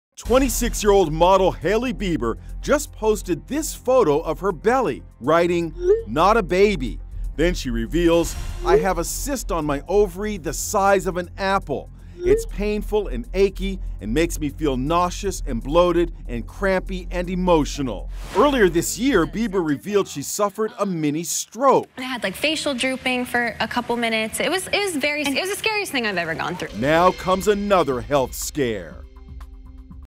Hailey Bieber Says She Has 'Painful' Apple-Sized Ovarian Cyst
Hailey Bieber recently posted a photo of her belly, writing, "Not a baby." The 26-year-old model revealed she has a painful cyst on one of her ovaries.